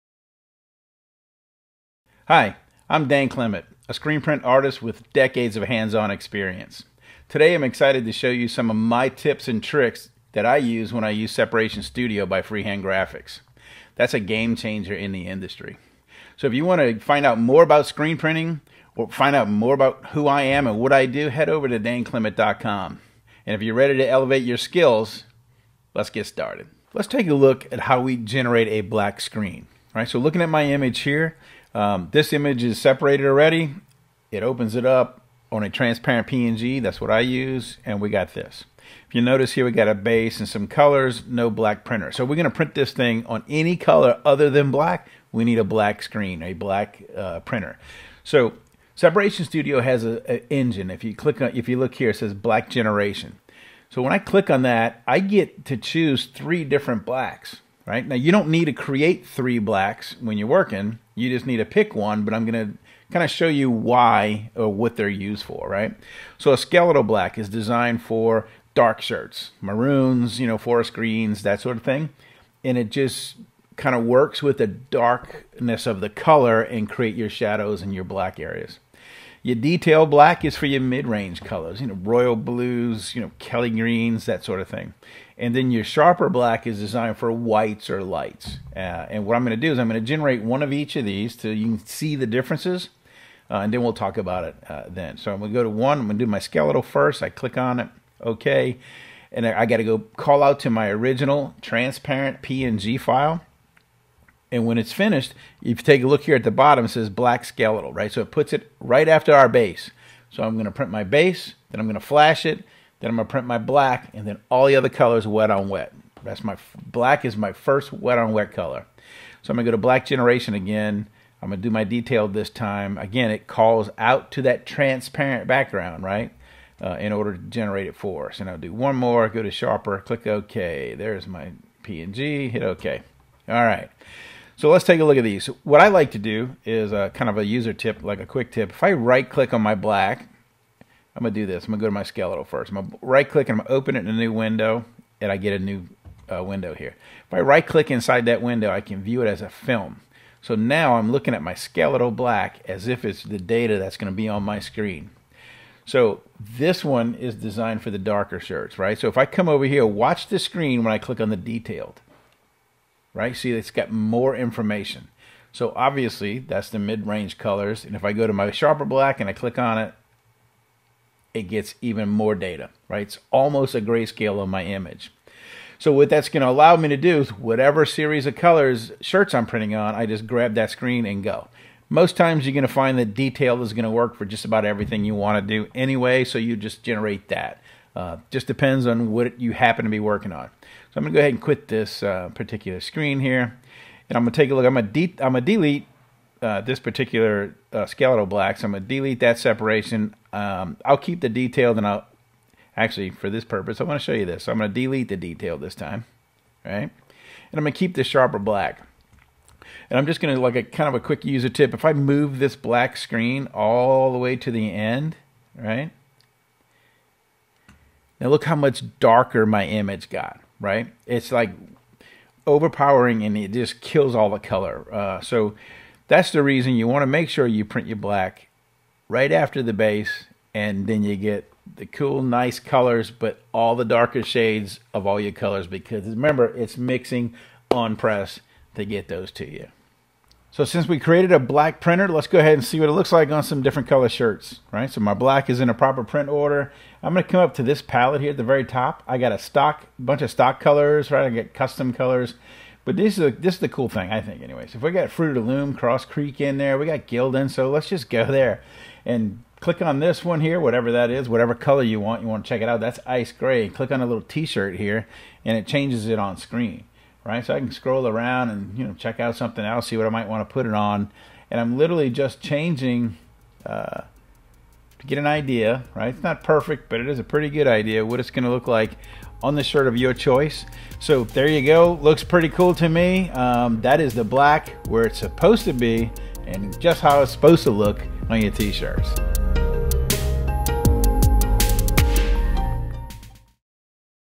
Separation Studio NXT - Print Like You've Got a Big Press - Black Generation - with Dane Clement
Legendary Artist and Screen Printer Dane Clement does a walkthrough of how to use the Black Generation feature of Sep Studio NXT so that you can print like you've got a big press.